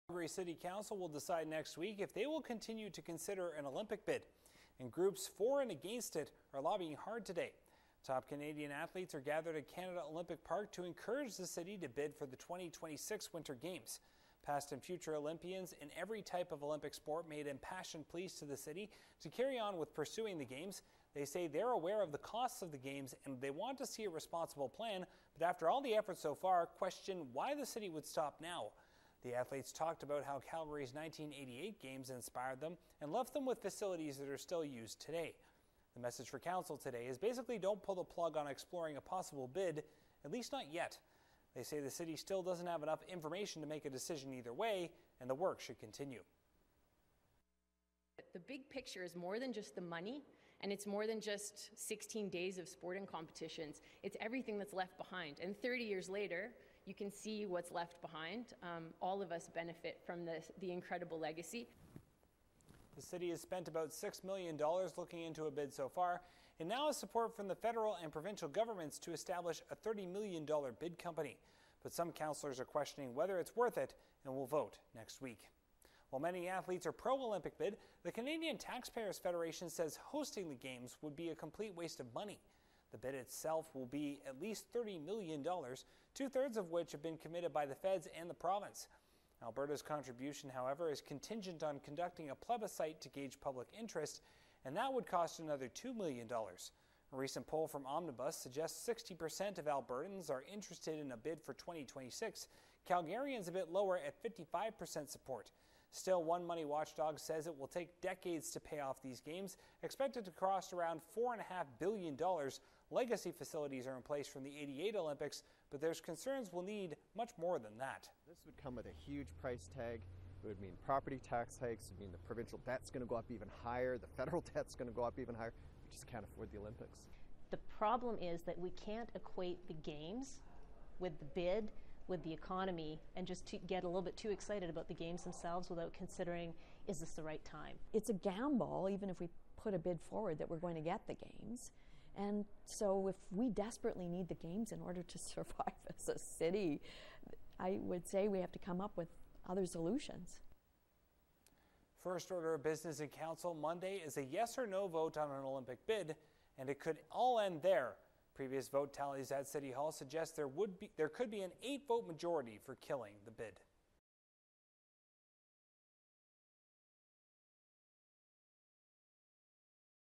Heated battle over Calgary Olympic bid
With Calgary city hall being petitioned on both sides by those for and against an Olympic bid, CTF Alberta Director Colin Craig says City council should say no. The costs for hosting the 2016 Winter Olympics are simply too high and will saddle Calgarians with higher property taxes for years to come. Calgary city hall has already spent $6 million in just preparing to make a bid.